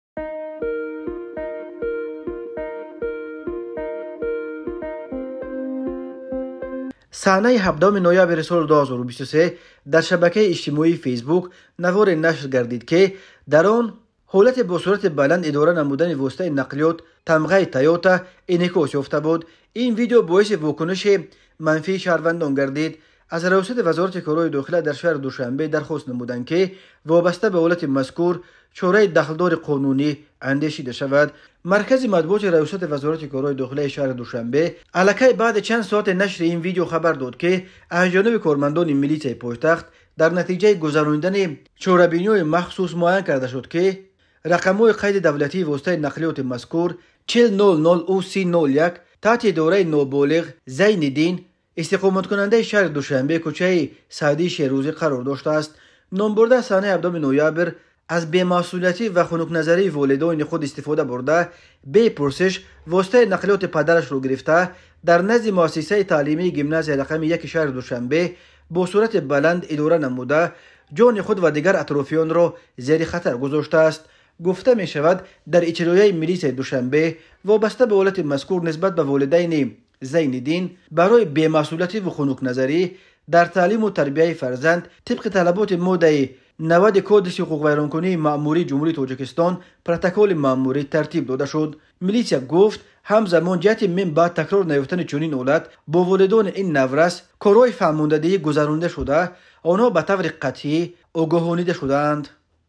ДАР ДУШАНБЕ ЯК НОБОЛИҒ БО МОШИНИ ПАДАРАШ БА МАКТАБ ОМАДААСТ
Санаи 17-уми ноябри соли 2023 дар шабакаи иҷтимоии Фейсбук наворе нашр гардид, ки дар он ҳолати бо суръати баланд идора намудани воситаи нақлиёти тамғаи «Тойота» инъикос ёфта буд. Ин видео боиси вокунишҳои манфии шаҳрвандон гардида, аз Раёсати ВКД дар шаҳри Душанбе дархост намуданд, ки вобаста ба ҳолати мазкур чораи дахлдори қонунӣ андешида шавад.
Маркази матбуоти РВКД Душанбе алакай баъди чанд соати нашри видео хабар дод, ки аз ҷониби кормандони милитсияи пойтахт, дар натиҷаи гузаронидани санҷиш муайян карда шуд, ки рақамҳои қайди давлатии воситаи нақлиёти мазкур 40 00 UC 01 буда, таҳти идораи ноболиғ Зайниддини Ҷ. истиқоматкунандаи шаҳри Душанбе, кӯчаи Саъдии Шерозӣ қарор доштааст.
 “Номбурда санаи 17-уми ноябр аз бемасъулиятӣ ва хунукназарии волидайни худ истифода бурда, бе пурсиш воситаи нақлиёти падарашро гирифта, дар назди муассисаи таълимии Гимназияи №1-и шаҳри Душанбе бо суръати баланд идора намуда, ҷони худ ва дигар атрофиёнро зери хатар гузоштааст”, - гуфта мешавад дар иттилоияи милитсияи Душанбе.
  Вобаста ба ҳолати мазкур нисбат ба волидайни Зайниддини Ҷ. барои бемасъулиятиву хунукназарӣ дар таълиму тарбияи фарзанд, тибқи талаботи моддаи 90 Кодекси ҳуқуқвайронкунии маъмурии Ҷумҳурии Тоҷикистон протоколи маъмурӣ тартиб дода шуд.
Милитсия гуфт, ҳамзамон ҷиҳати минбаъд такрор наёфтани чунин ҳолат, бо волидайни ин наврас корҳои фаҳмондадиҳӣ гузаронида шуда, онҳо ба таври қатъӣ огоҳонида шудаанд.